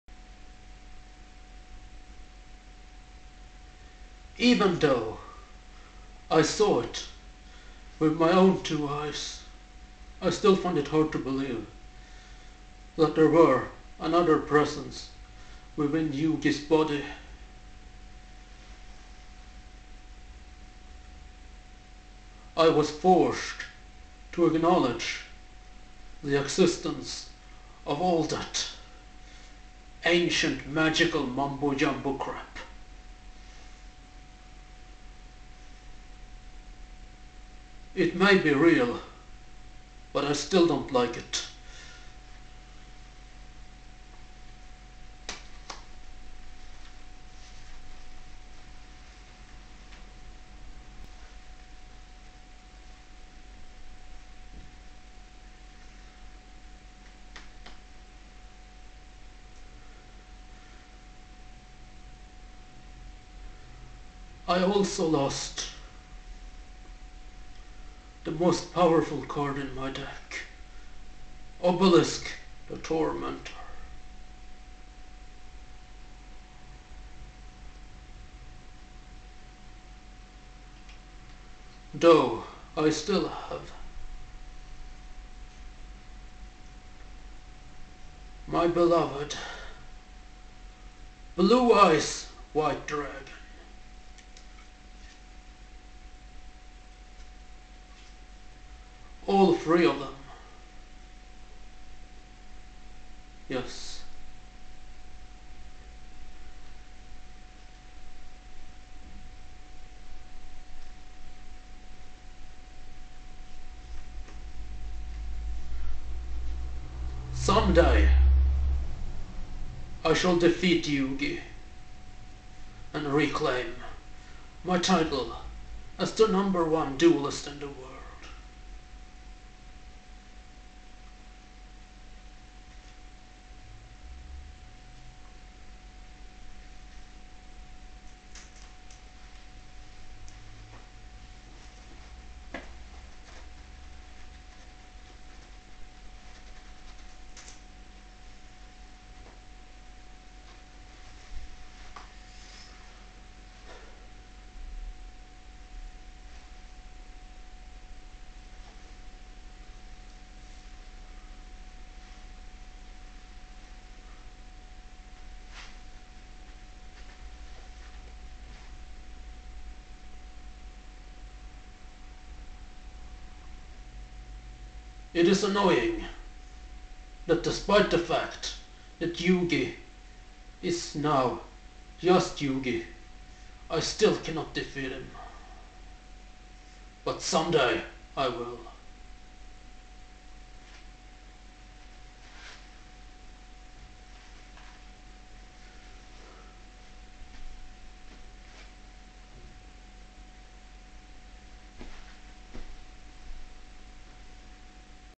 Seto Kaiba.
Seto Kaiba is back again here on my channel. This was filmed just a week after the Ceremonial Battle between Pharao Atem and Yugi. Kaiba admit that the magic of the past is real, but dislike this fact very much.